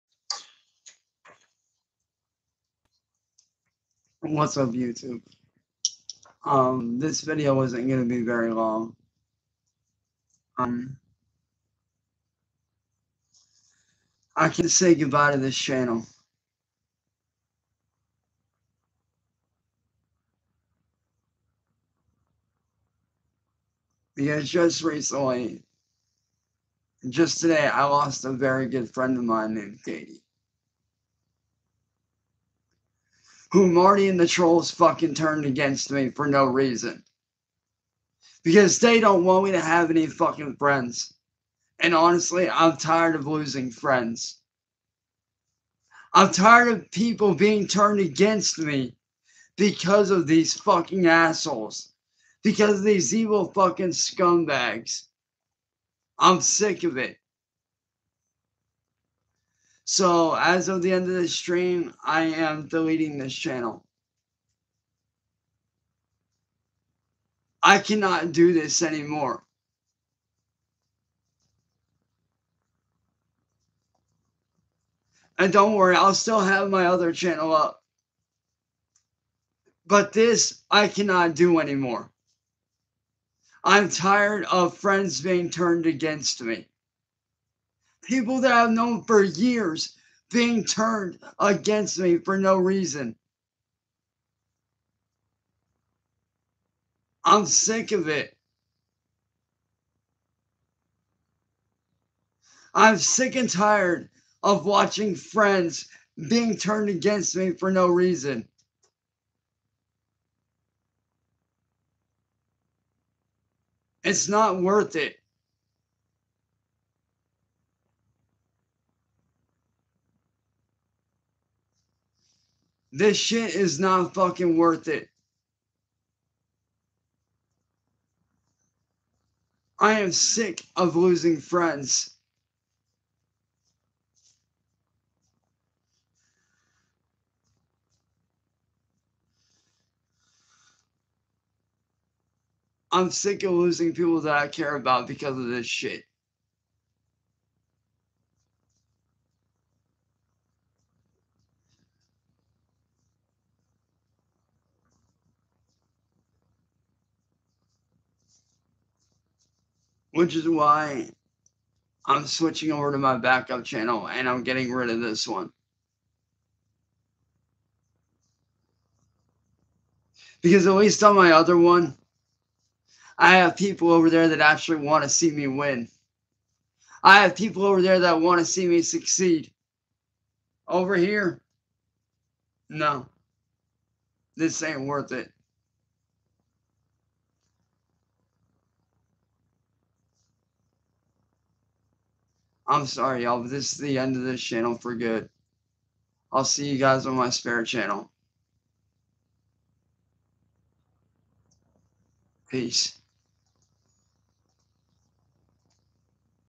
Cyraxx live on YT. "IM LEAVING THIS CHANNEL BEHIND FOR GOOD !!!!!!!!!!!!!!!". 6/1/2024. Poor PDF.
Upload from "his" YouTube channel 30 minutes ago. This was a livestream.